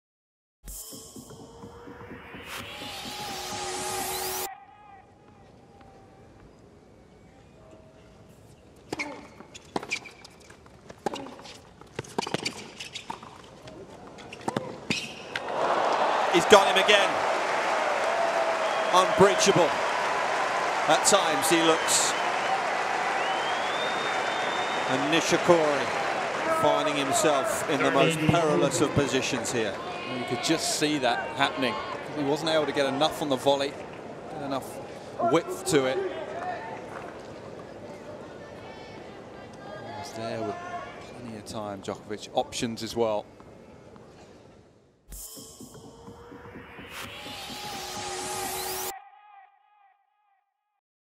Djokovic Finds The Angle Miami 2016 Hot Shot
Watch Hot Shot as Novak Djokovic hits a devastating forehand pass against Kei Nishikori. Watch live tennis at tennistv.com.